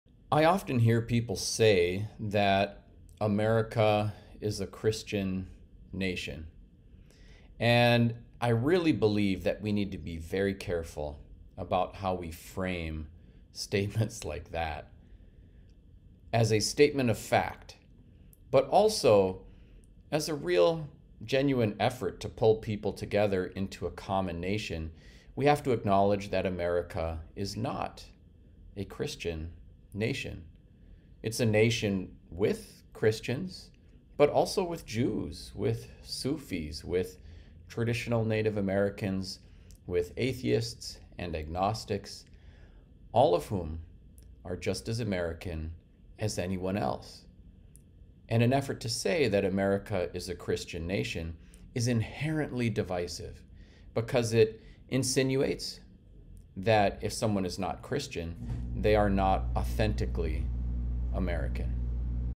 American Is Not A Christian Nation
Anton Treuer shares a perspective about how we frame religion in American political and social spaces. We are not a Christian nation, we are a nation with Christians.